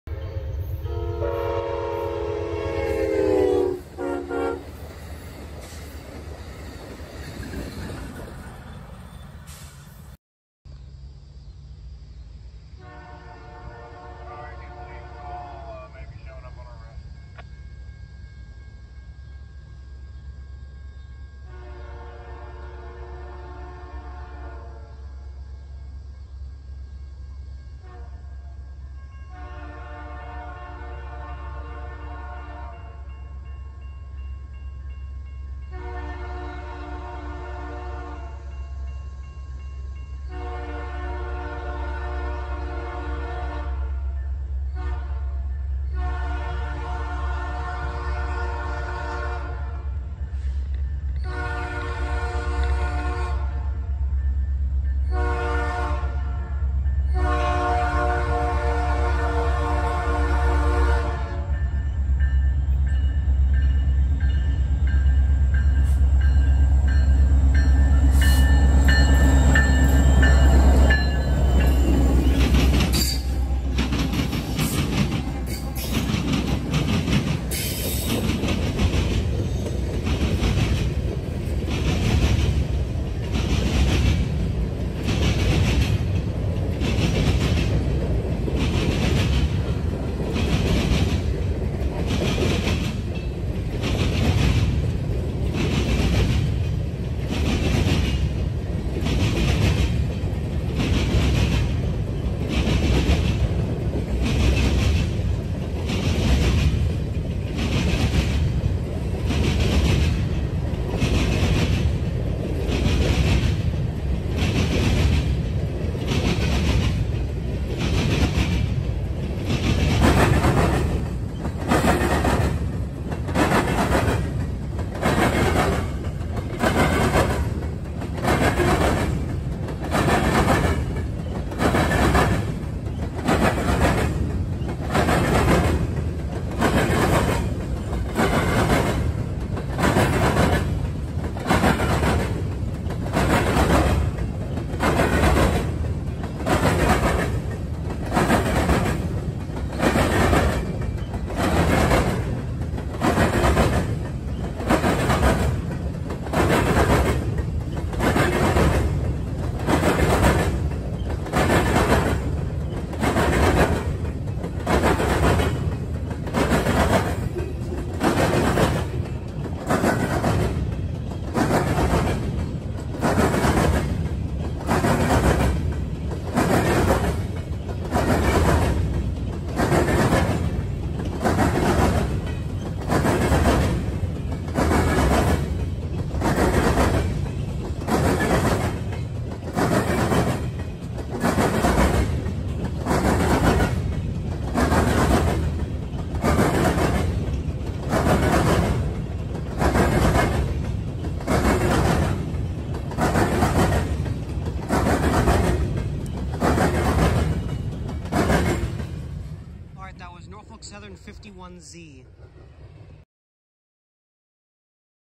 New Intro! NS 1160 with Fouled P5 leads 51Z and pounds through the Bucyrus Ohio Diamond.
Hope the intro is okay! Short and simple but hopefully affective.